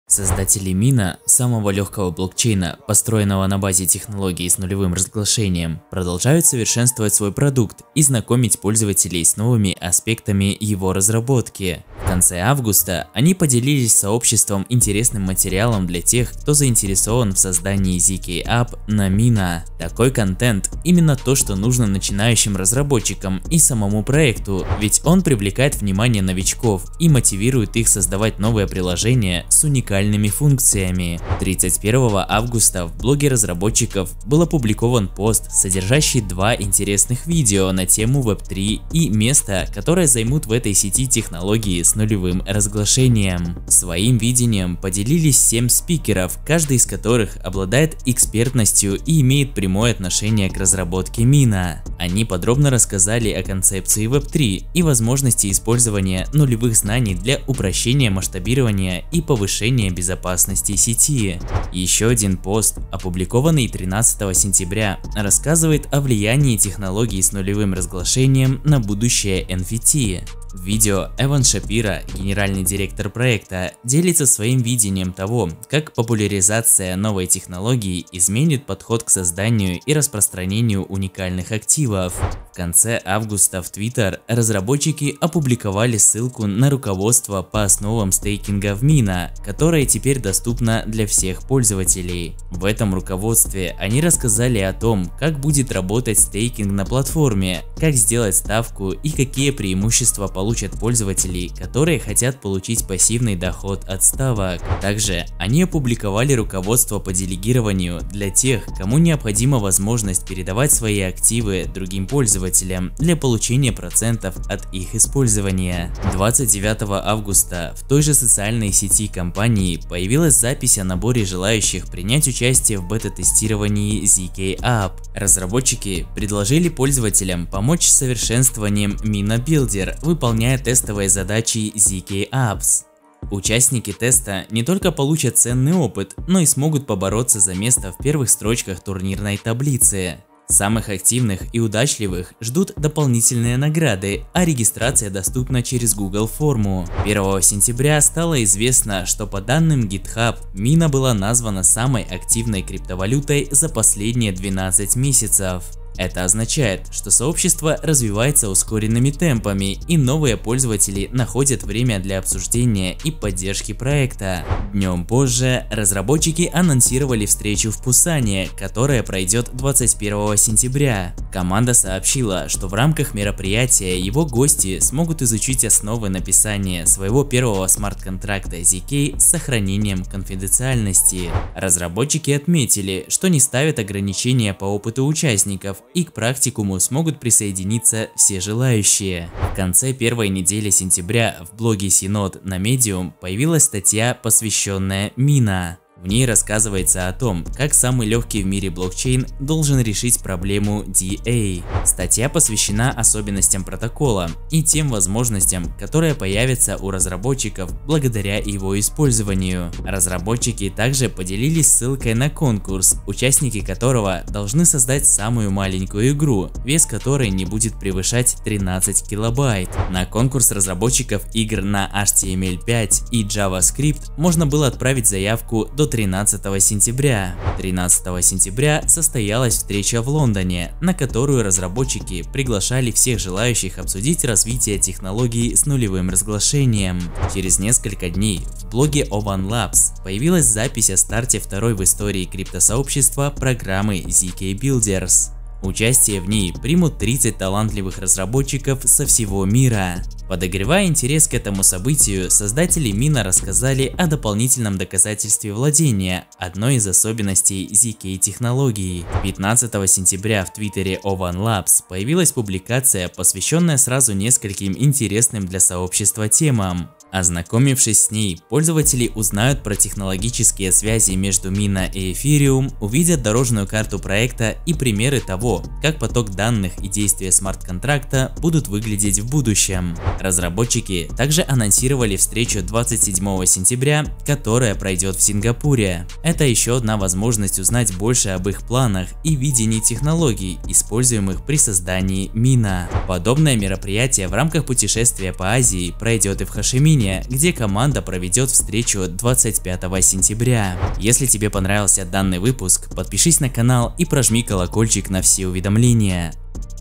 Mina Protocol новости | MINA FOUNDATION | O(1) LABS | MINA Web3 | zk Builders | Ethereum pos
Создатели Mina – самого легкого блокчейна, построенного на базе технологии с нулевым разглашением, продолжают совершенствовать свой продукт и знакомить пользователей с новыми аспектами его разработки. В конце августа они поделились с сообществом интересным материалом для тех, кто заинтересован в создании zkApp на Mina. Такой контент – именно то, что нужно начинающим разработчикам и самому проекту, ведь он привлекает внимание новичков и мотивирует их создавать новые приложения с уникальными функциями.